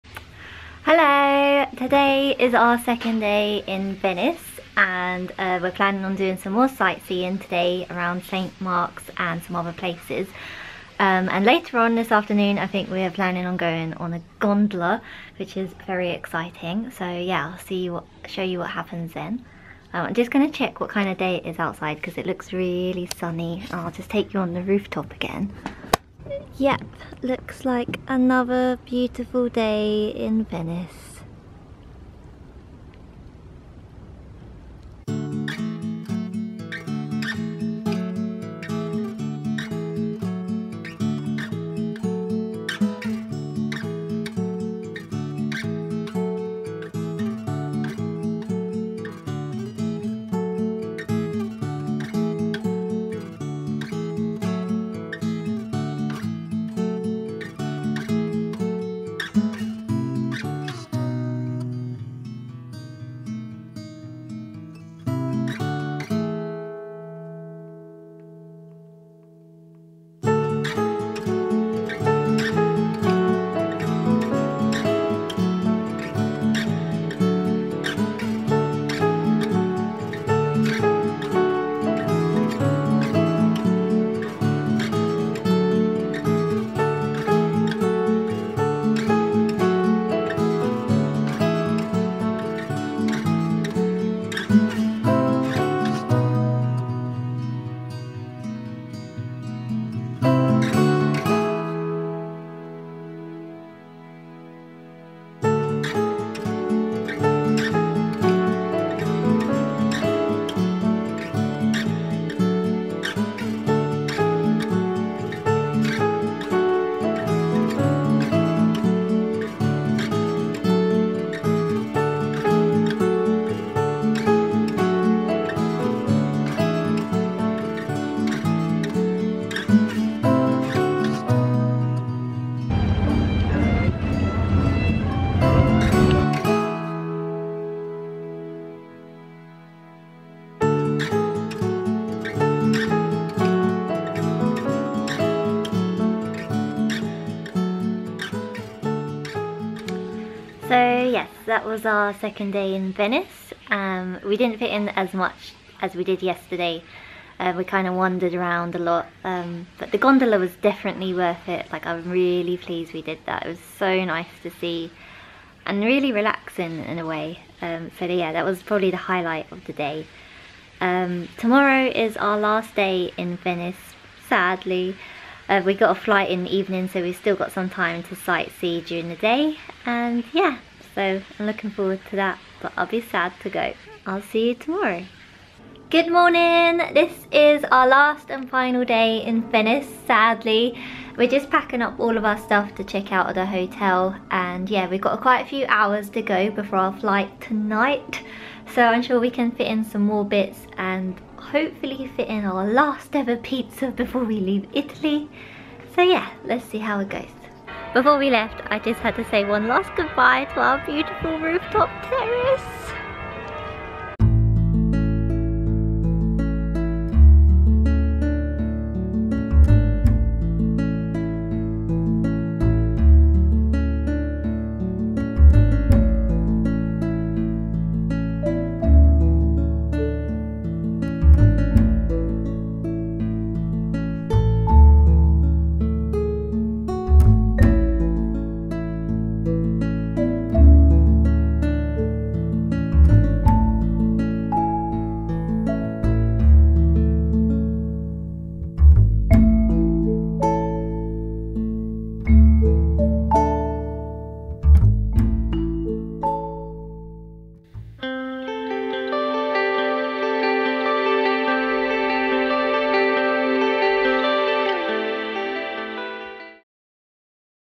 Venice Vlog • Day 2 + 3 • Gondola Ride & More Venice • Emma Rose
Here's the second part to my Venice vlog.  We did more exploring on our second and final day around San Marco and further afield in Dorsoduro and San Polo.  Unfortunately filming in many of the beautiful churches was not allowed, but I hope you still enjoy our wanderings.